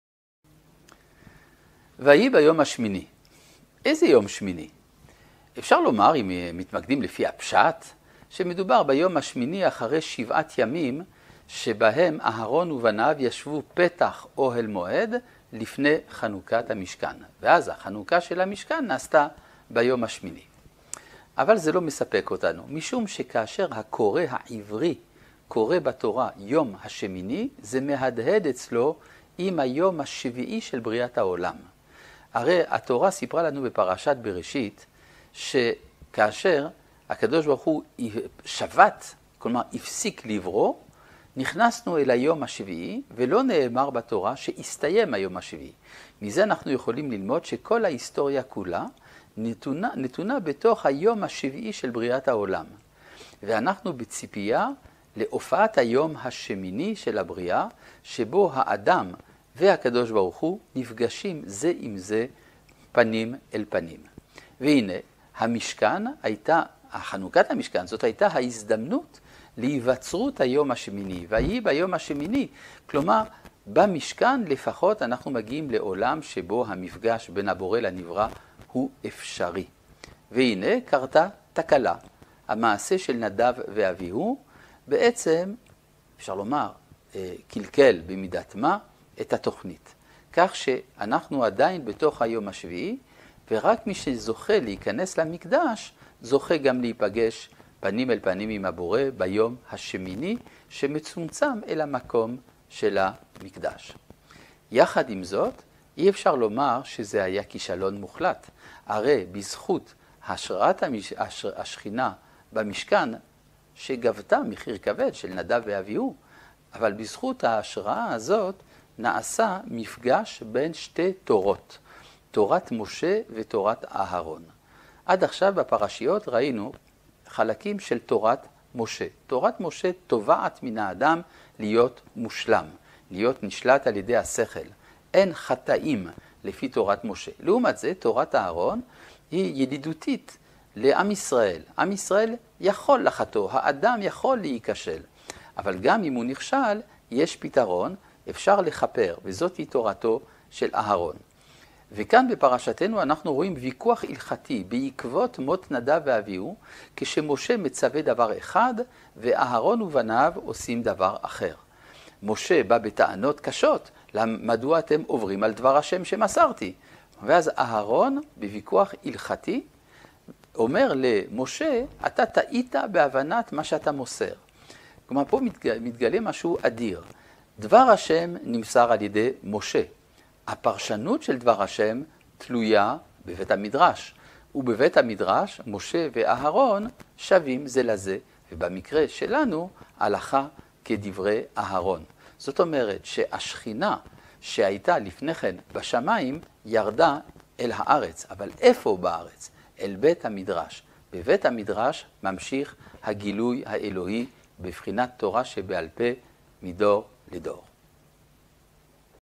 26 | Смерть Надава и Авиѓу сделала возможной встречу Учения Моше и Учения Аѓарона, раздел Шмини
«И было, на восьмой день». О каком восьмом дне здесь идет речь? В соответствии с прямым смыслом Писания можно было бы сказать, что имеется в виду восьмой день после тех семи дней, когда Аѓарон и его сыновья сидели перед Шатром Откровения перед освящением Мишкана (Скинии). И, в соответствии с таким пониманием, освящение Мишкана происходило на восьмой день. Однако, такое понимание этой фразы нам кажется недостаточным, поскольку слова «на восьмой день» напрямую ассоциируются у нас с седьмым днем Творения и требуют дополнительного объяснения. В разделе Берешит Тора рассказывает нам, что, когда Святой, Благословен Он, закончил творить, мы вступили в седьмой день, при этом об окончании этого дня не сказано. И из этого мы учим, что вся человеческая история располагается в седьмом дне творения мира и мы находимся в ожидании восьмого дня творения, когда человек сможет непосредственно встретить  своего Творца.
Освящение Мишкана как раз и представляло такую возможность возникновения восьмого дня и сказанное «И было, на восьмой день» можно понимать, что именно во время освящения Мишкана встреча с Творцом стала возможна.
Но вот происходит трагедия, Надав и Авиѓу погибают, и их смерть, можно сказать, «нарушила» первоначальный план, и мы продолжаем оставаться в седьмом дне и только тот, кто удостаивается войти в Храм, удостаивается встречи с Творцом в восьмой день. И этот день сокращается до пространства Храма.
Вместе с тем, нельзя сказать, что это был полный провал, ведь с воцарением Божественного присутствия (Шхины) в Мишкане, что было связано с тяжелой утратой жизней Надава и Авиѓу, стала возможной встреча двух Учений – Учения Моше и Учения Аѓарона.
До сих пор мы видели в предыдущих разделах только представление Учения Моше, которое требует от человека быть совершенным и поступать исключительно из соображений разума, требуя оставатся при этом безгрешным. Тогда, как Учение Аѓарона выступает благосклонным в отношении народа Израиля, предполагая возможность совершения человеком греха, возможность проступка и ошибки, оставляя возможность искупления содеянного. И в этом смысл Учение Аѓарона.
Дополнительно, здесь, в этом недельном разделе, в результате трагической гибели Надава и Авиѓу,  мы становимся свидетелями ѓалахического спора между Моше и Аѓароном, когда Моше заповедует одно, а Аѓарон и его оставшиеся сыновья исполняют другое. Моше обращается с тяжелыми обвинениями в адрес Аѓарона – «Почему вы нарушаете то, что я передал вам от Имени Всевышнего?» И тогда Аѓарон в ѓалахическом споре с Моше убеждает его, что он неправильно понял переданное ему сообщение. То есть, мы видим здесь удивительную вещь: слово Всевышнего передано через Моше, однако толкование этого слова зависит от решения Бейт-Мидраша (Дома Учения), где Моше и Аѓарон равны в своих мнениях, и, в нашем конкретном случае, ѓалаха устанавливается по мнению Аѓарона. То есть, Шхина, Божественное присутствие, которое пребывало до этого на Небесах, теперь спустилась на землю. Но где же на земле? В Бейт-Мидраше (в Доме Учения), продолжается Божественное Откровение в форме Устной Торы, передающейся из поколения в поколение.